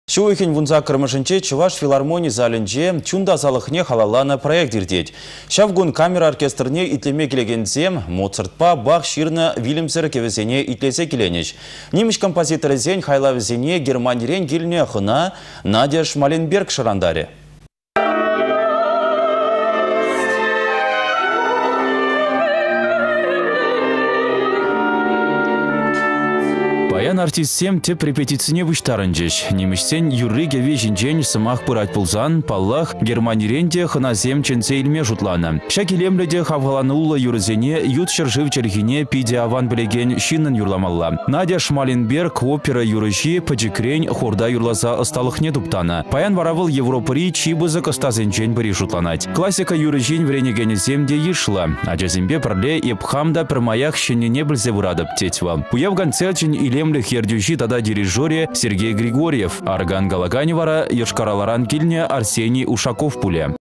Шупашкарта «Экология души» проект пуçару илет
Çу уйăхĕн 18-мĕшĕнче Чăваш филармони залĕнче Чун тасалăхне халалланă проект иртет. Çав кун камера оркестрне итлеме килекенсем Моцартпа Бах çырнă вилĕмсĕр кĕвĕсене итлесе киленĕç. Нимĕç композиторĕсен хайлавĕсене Германирен килнĕ хăна Надя Шмаленберг шăрантарĕ.